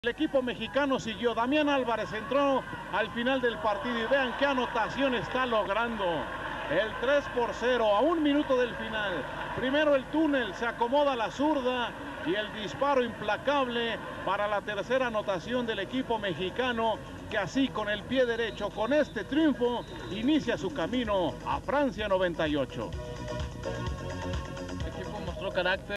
Damian Alvarez Arcos Debuta con la Selección Nacional de México con un golazo.
Damian Álvarez Arcos Anota Golazo con la Selección de México en su Debut.